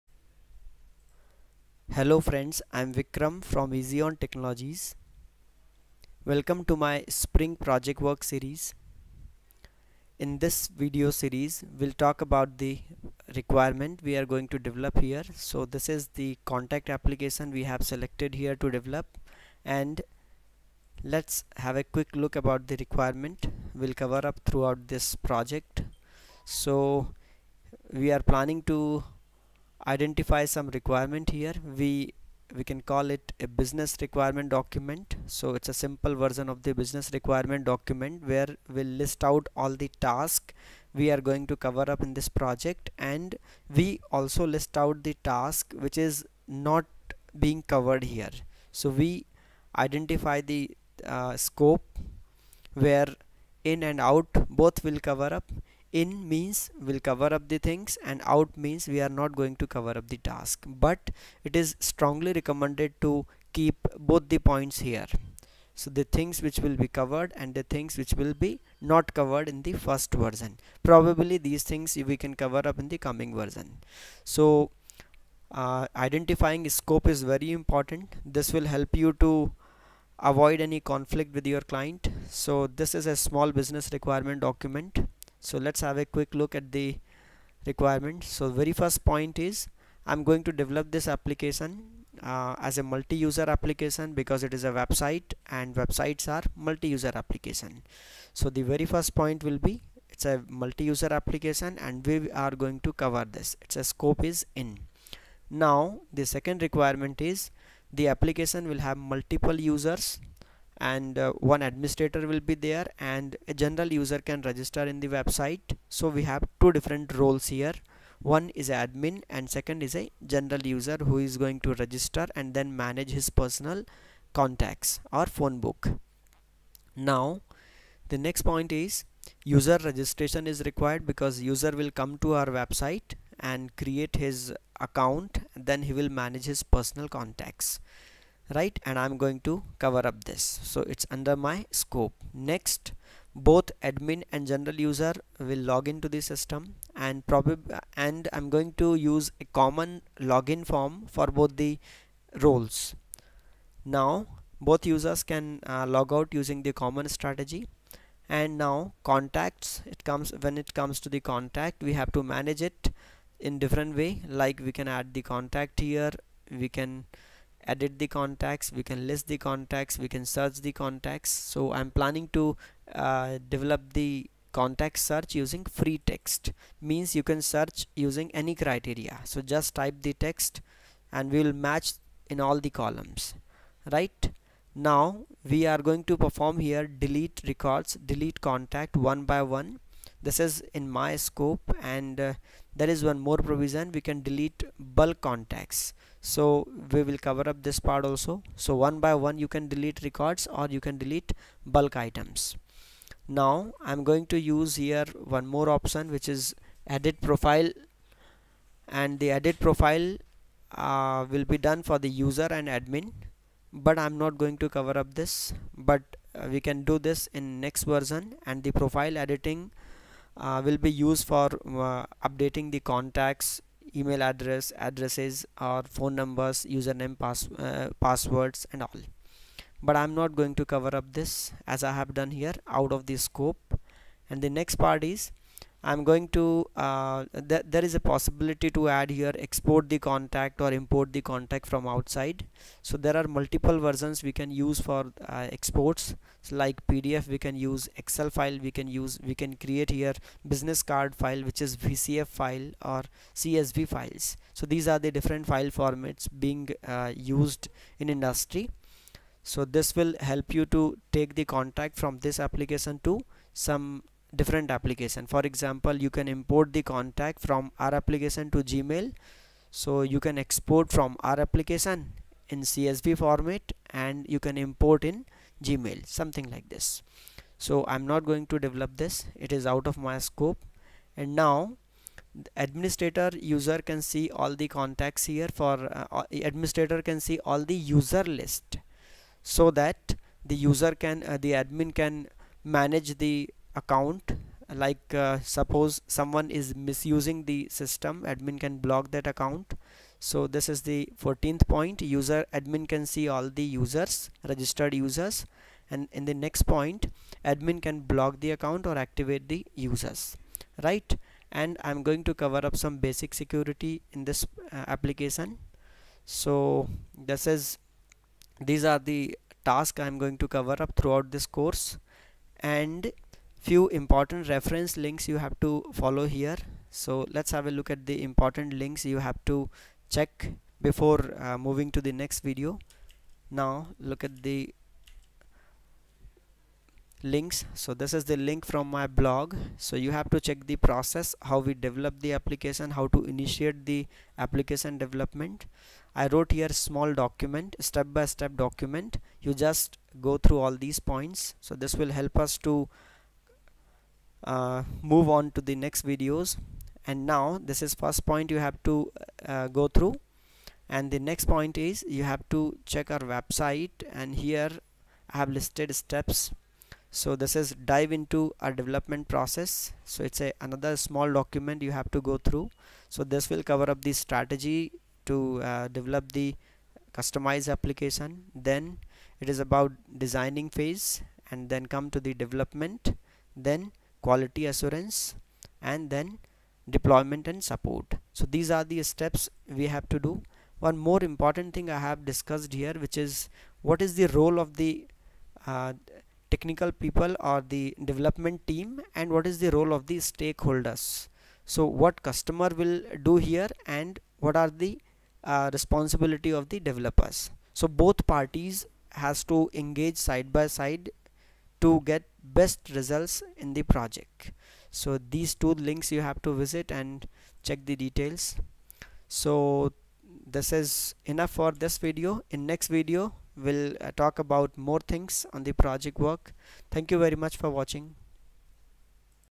2 - Spring MVC - Project Work - Requirement (Contact App) - Java Training By eZeon
This Project Work(Contact App) video discussed about the project requirement(Business Requirement Document - BRD).

Do not skip or miss any video, just follow step by step videos.
Also check the source code listing in our